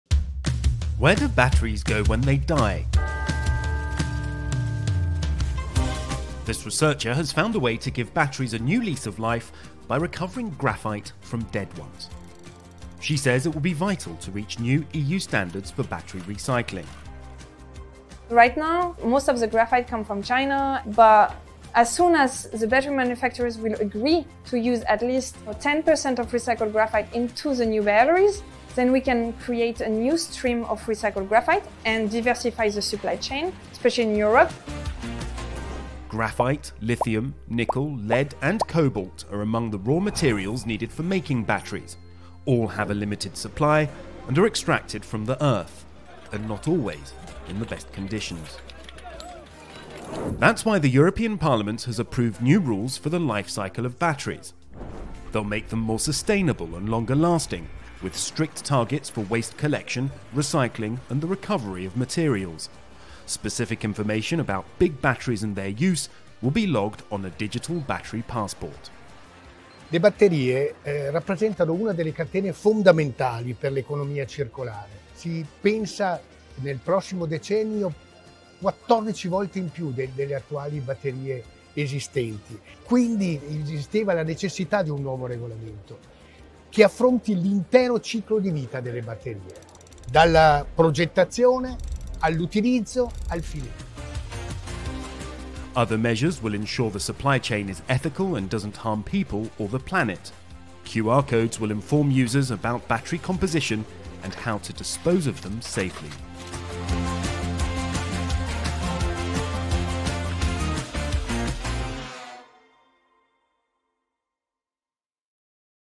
Circular economy: getting more life from batteries
Phones, computers, electric cars… they all use batteries. And demand is increasing rapidly.

The European Parliament has approved new rules to reduce the impact on the environment, ensuring minimum standards for recycling batteries and their component materials.